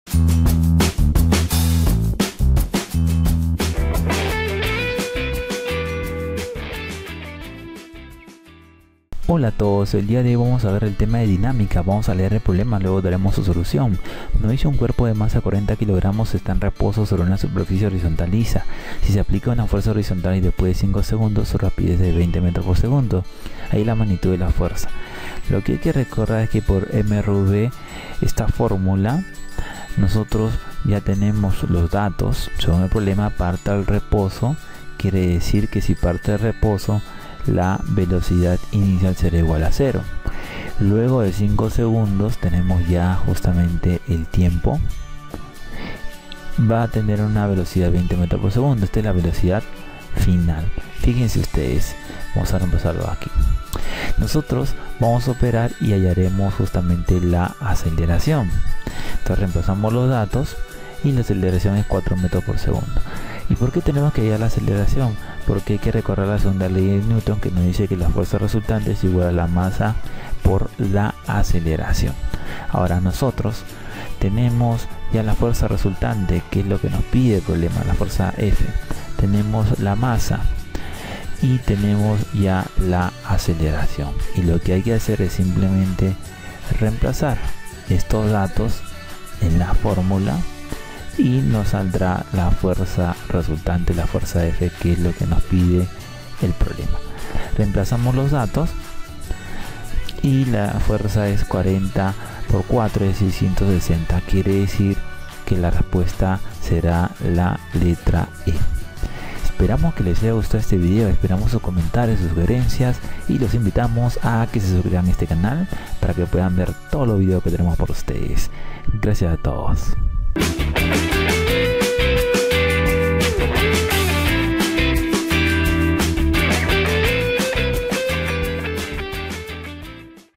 Cómo resolver un problema de dinámica - 2da ley de Newton(BIEN EXPLICADO, PASO A PASO)
En este vídeo se explica cómo resolver un problema de Dinámica en el curso de Física. Este ejercicio fue tomado en un examen del Centro Preuniversitario de la Universidad Nacional Mayor de San Marcos de Perú y cumple los standar de calidad internacional. Por eso puede venir en exámenes de PSU, ENES, Saber 11, etc.